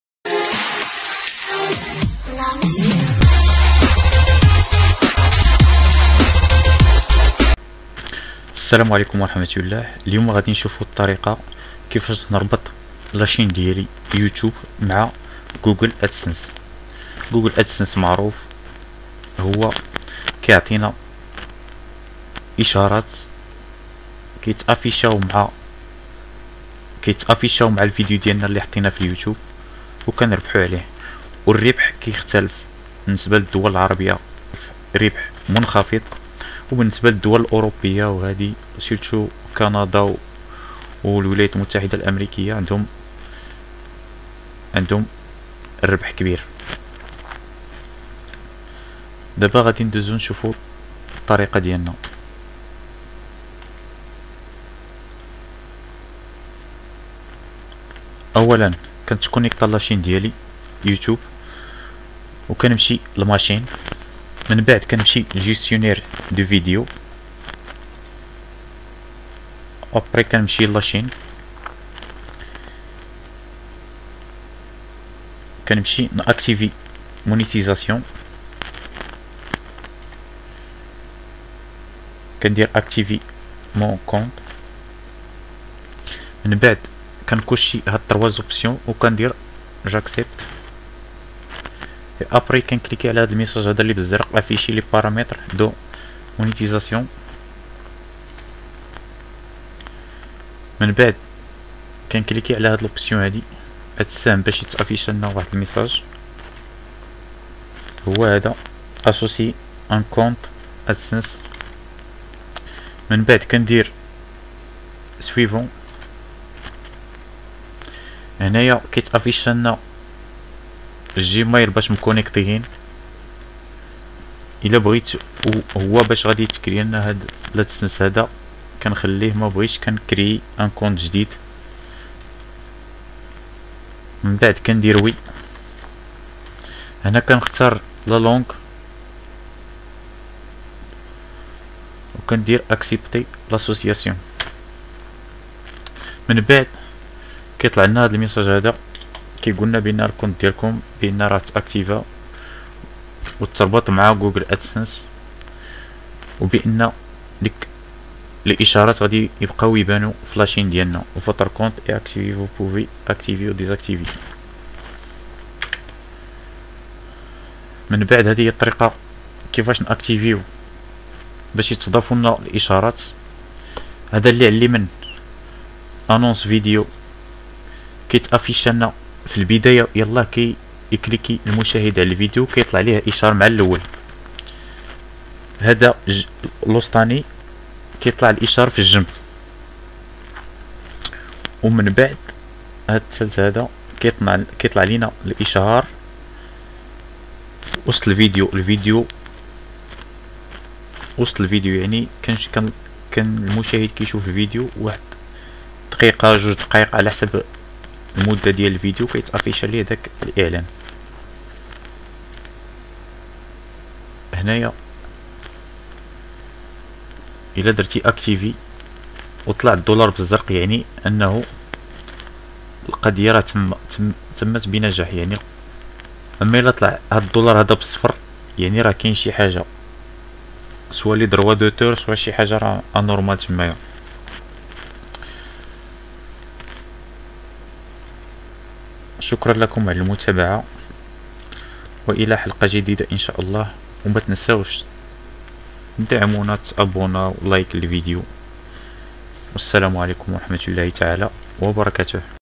طريقة ربط اليوتيوب بجوجل ادسنس | relié youtube avec google adsense
creation d'un compte adsense et l'associer a une chaine youtube